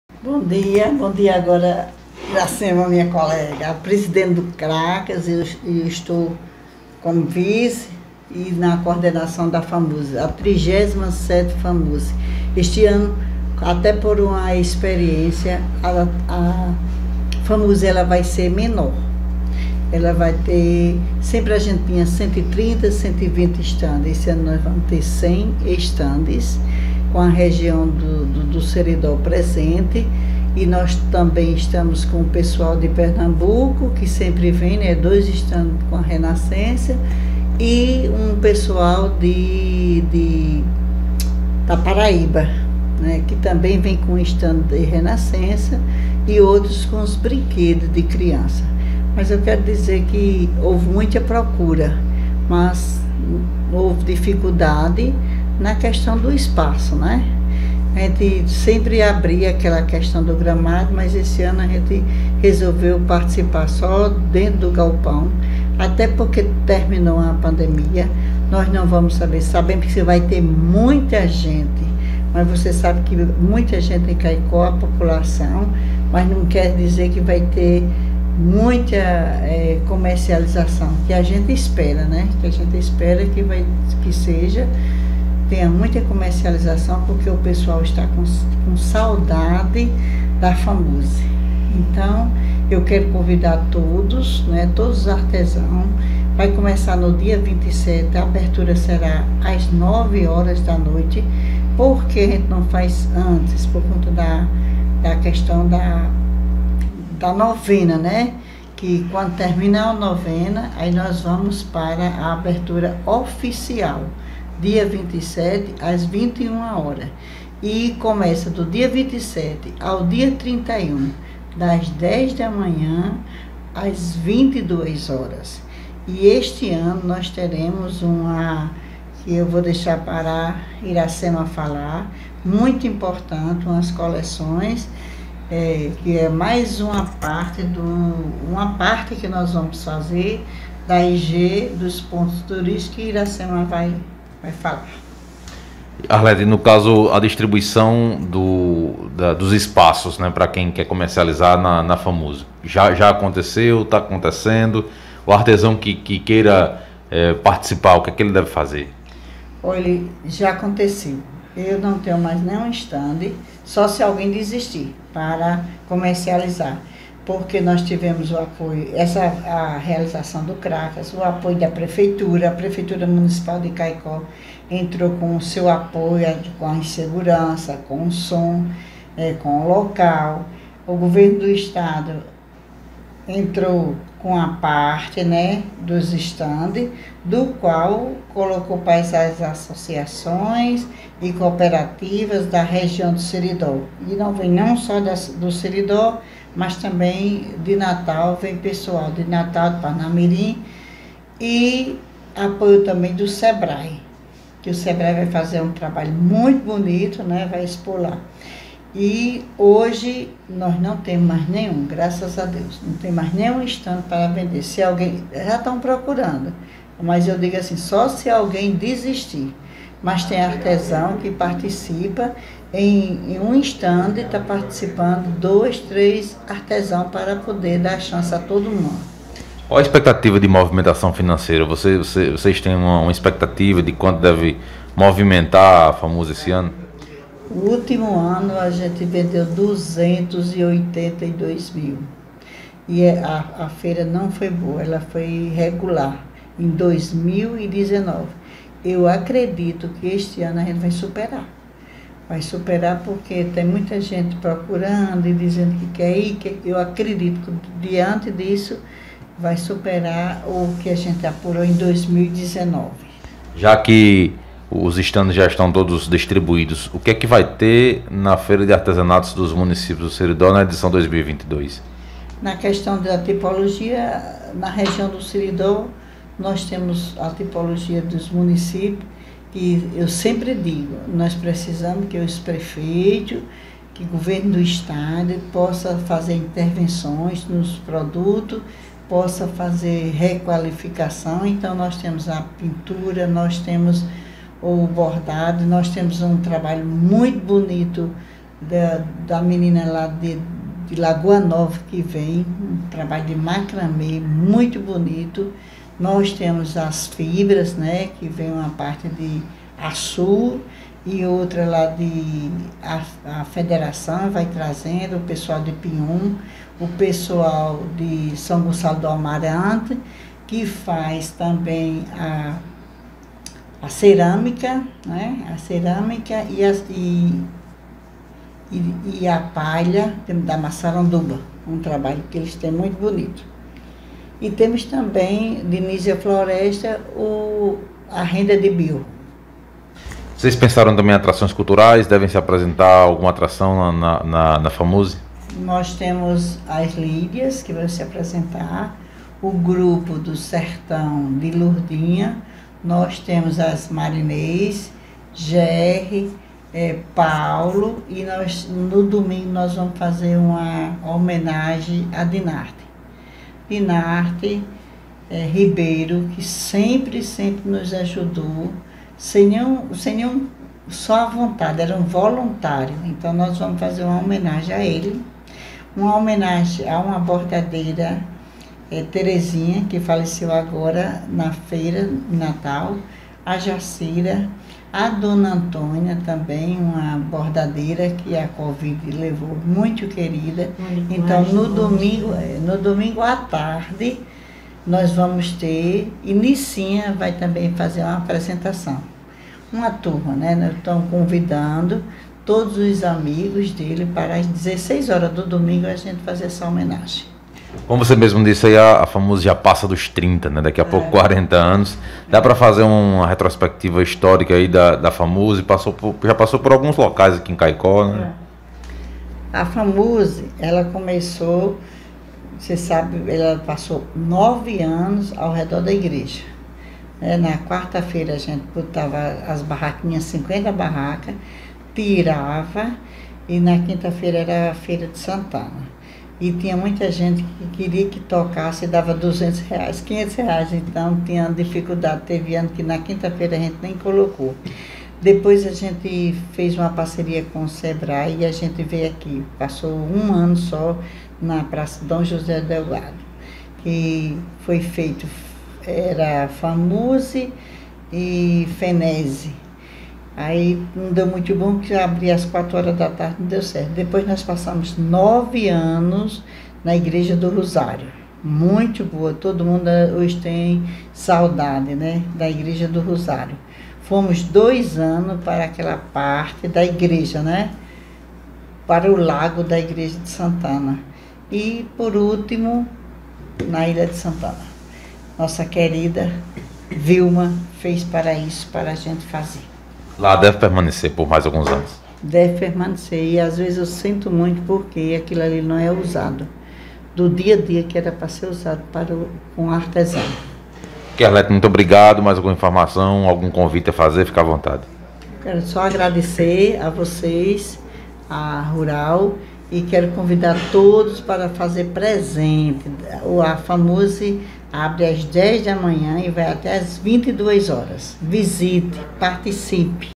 Tudo pronto para a Feira de Artesanato dos Municípios do Seridó
Os estandes da Feira de Artesanatos dos Municípios do Seridó - Famuse já estão todos comercializados. A informação foi confirmada por Arlete Silva, coordenadora do evento.